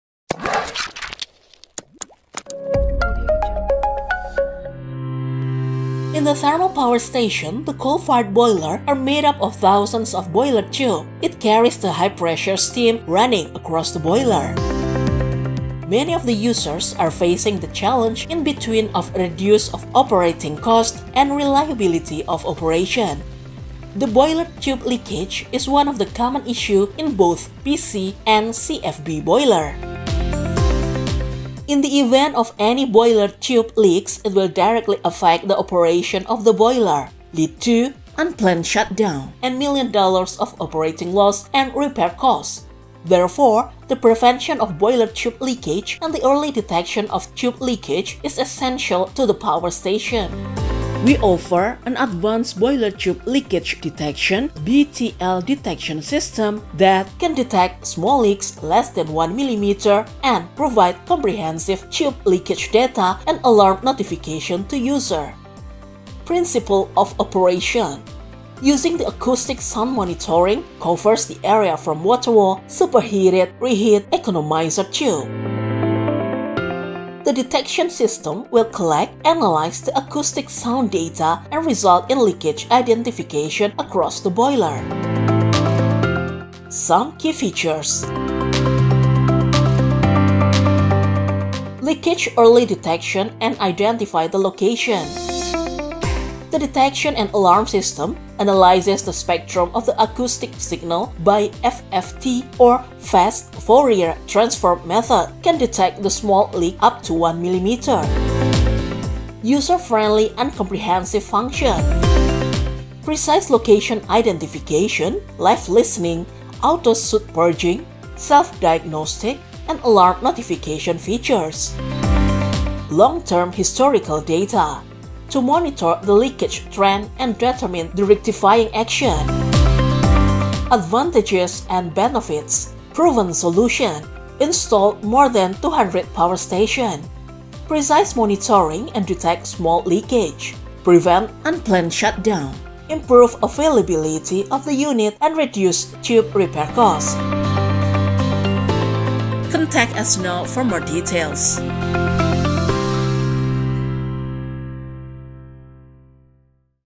Boiler Tube Leakage Detection (BTL Detection) System - EN
Boiler Tube Leakage Detection (BTL Detection) System

In the thermal power station, the coal fired boiler are made up of thousands of boiler tube. It carries the high pressure steam running across the boiler.

Many of the users are facing the challenge in between of reduce of operating cost and reliability of operation. The boiler tube leakage is one of the common issue in both PC and CFB Boiler.

We offer an advanced boiler tube leakage detection system that
• can detect small leaks less than 1mm &
•  provide comprehensive tube leakage data and alarm notification to user.

TECG Control Pte Ltd
140 PAYA LEBAR ROAD, #06-25 AZ @ PAYA LEBAR, SINGAPORE 409015.